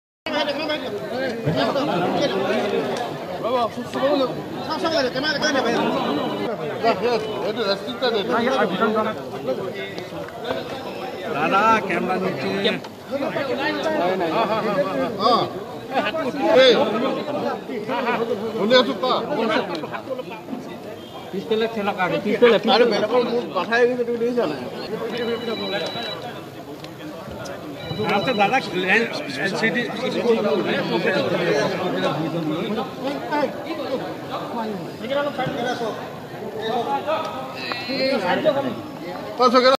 Assam Chief Minister Sarbananda Sonowal casts his vote at a polling center in Dibrugarh
Assam Chief Minister Sarbananda Sonowal in the first phase of Assembly elections on Saturday cast his vote at a polling centre in Dibrugarh. “We will get more than 100 seats,” said the Chief Minister after exercising his franchisee.